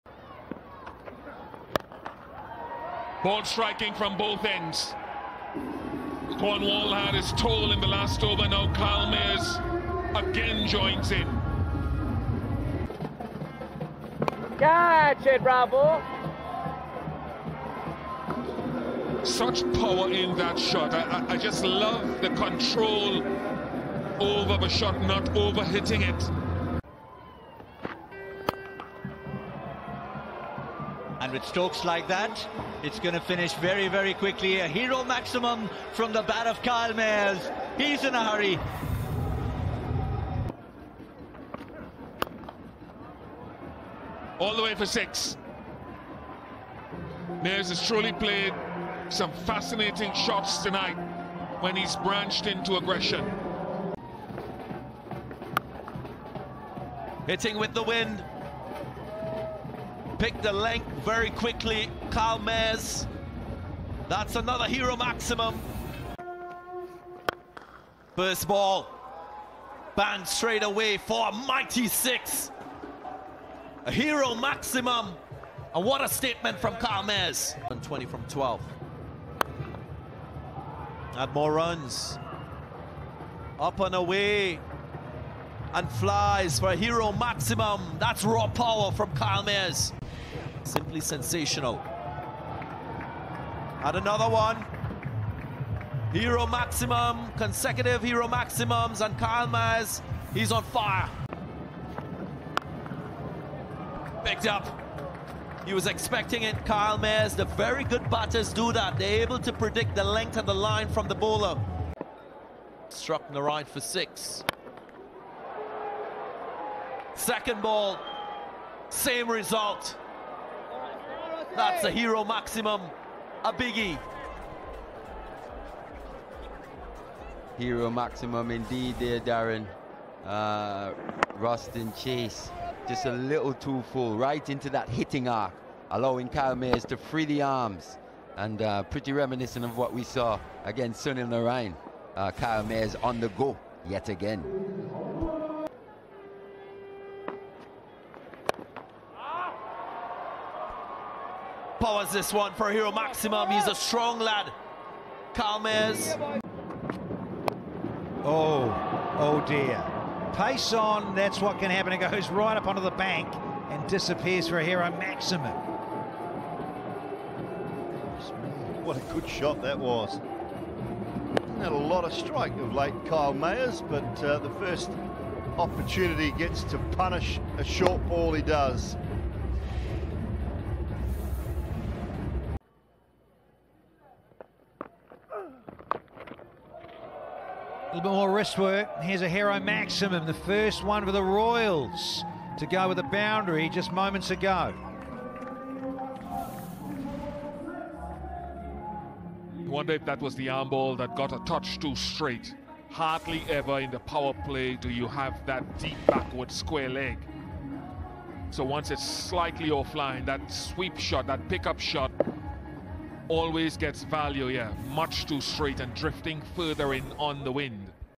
Kyle Mayers SMASHES it out of the park for fun! | CPL 2022
Kyle Mayers of the Barbados Royals certainly has no problem clearing the boundary rope. Enjoy all of the batsman's sixes from the CPL 2022 so far!